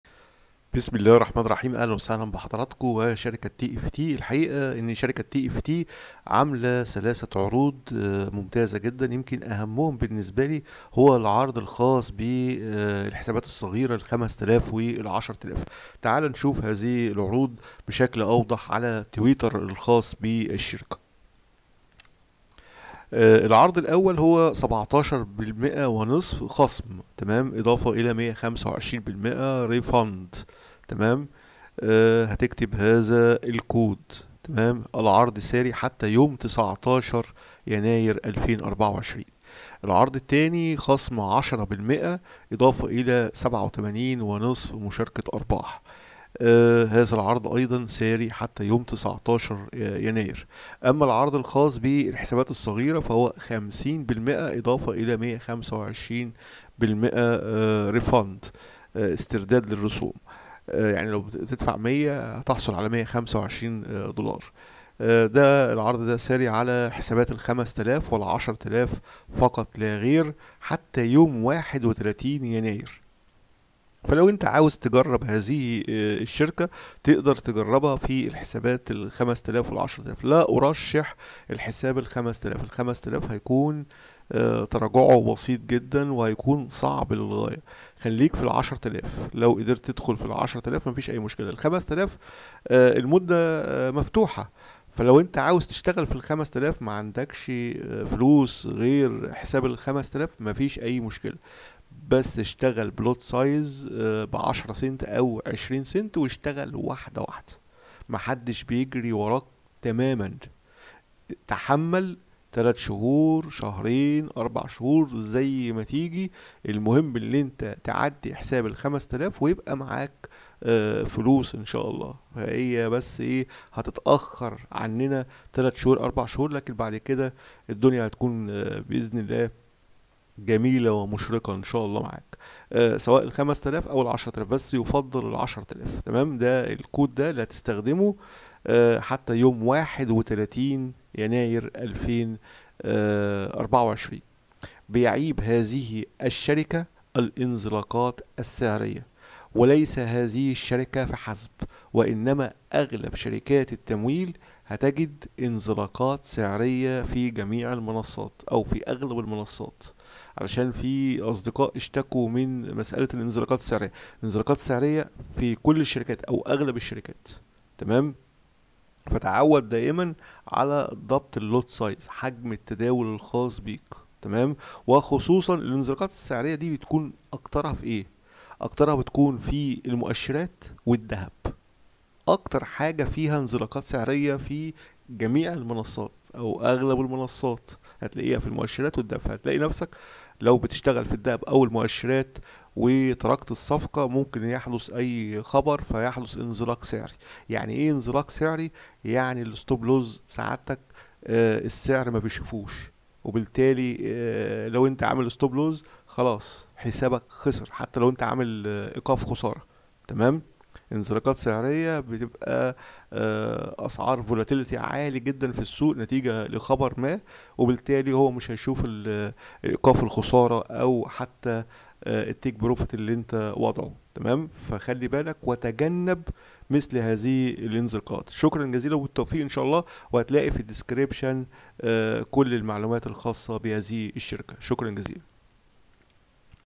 عروض يناير والحسابات الصغيرة فى شركة thefundedtraderprogram للتمويل في سوق الفوركس
للتسجيل في شركة thefundedtraderprogram للتمويل في سوق الفوركس

Code: TFTJANUARY1
End Date: 19th January

Code: TFTJANUARY50
End Date: 31st January